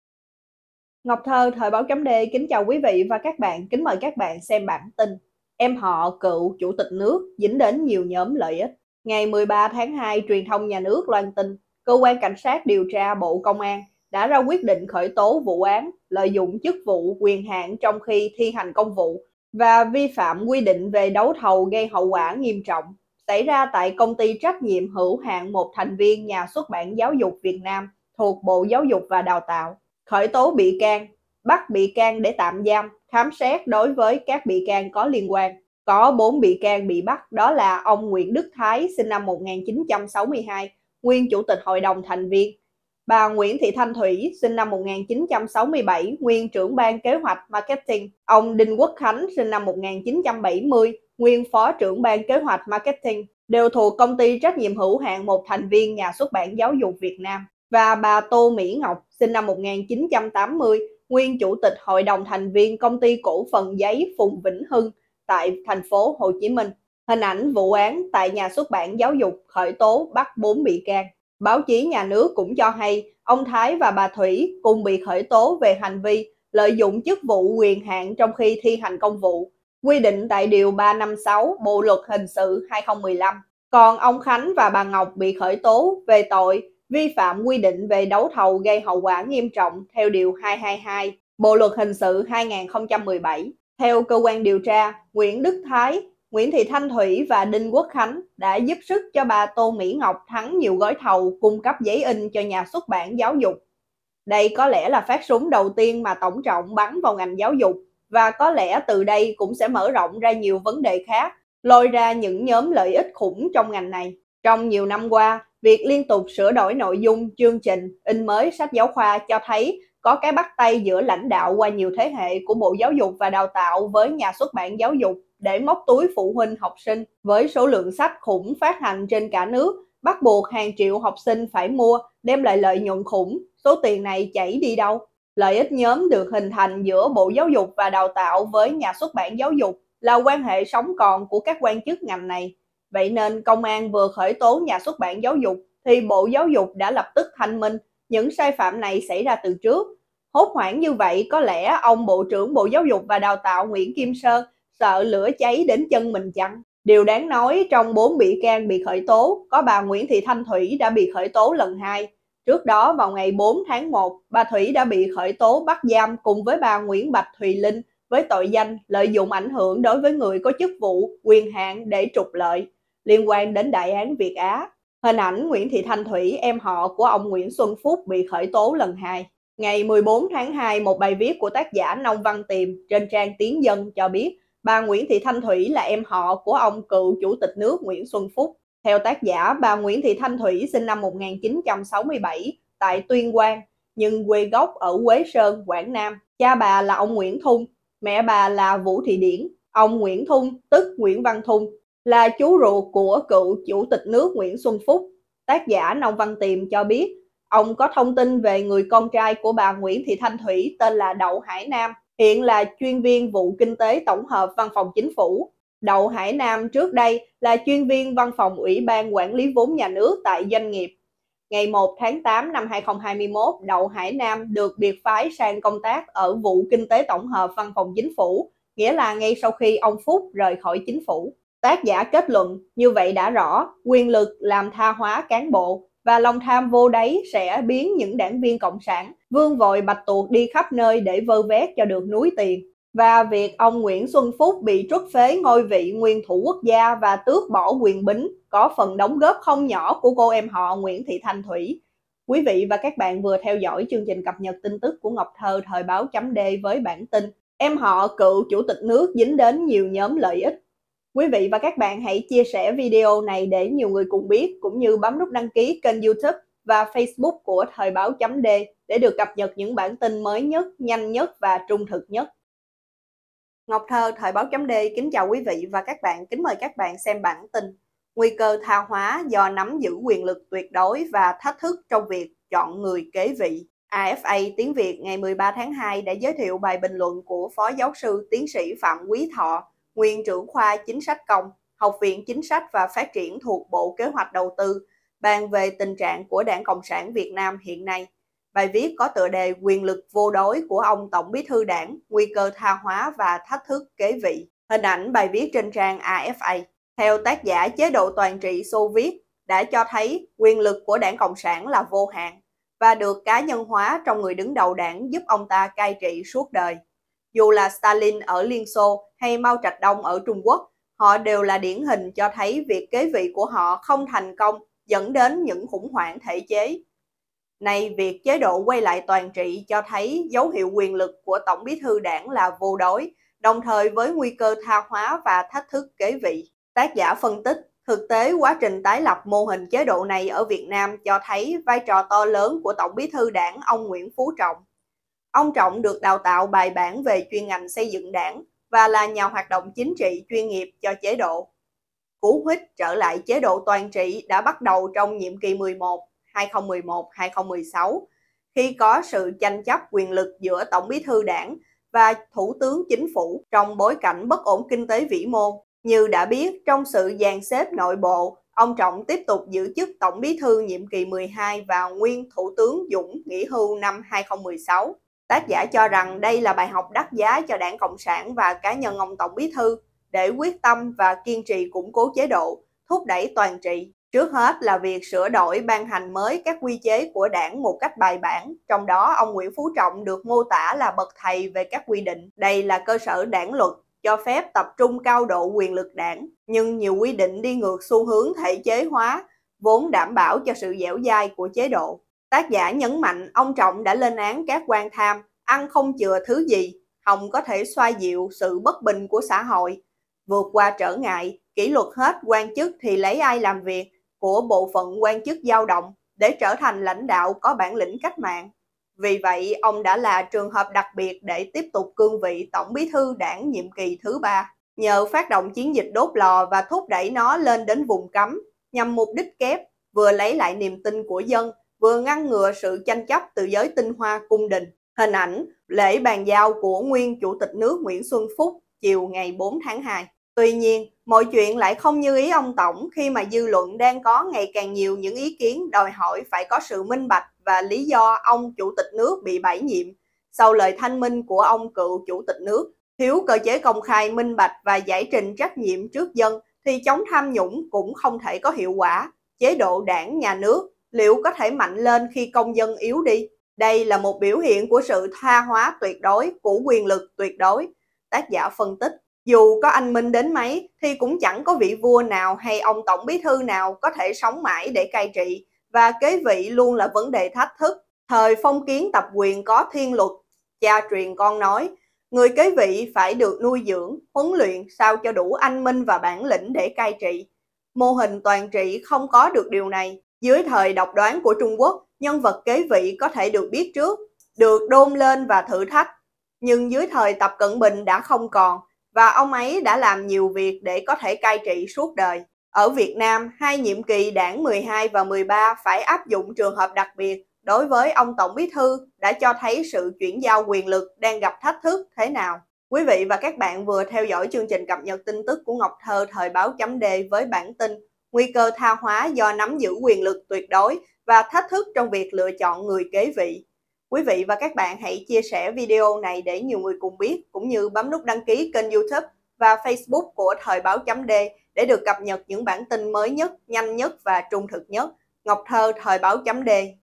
Em họ Phúc cựu Chủ tịch bị giật „Kim bài“ miễn xử
Quyền lực làm tha hóa cán bộ, và lòng tham vô đáy sẽ biến những đảng viên Cộng sản vươn “vòi bạch tuộc” đi khắp nơi để vơ vét cho được núi tiền. Việc ông Nguyễn Xuân Phúc bị truất phế ngôi vị nguyên thủ quốc gia và tước bỏ quyền bính, có phần “đóng góp” không nhỏ của cô em họ Nguyễn Thị Thanh Thuỷ.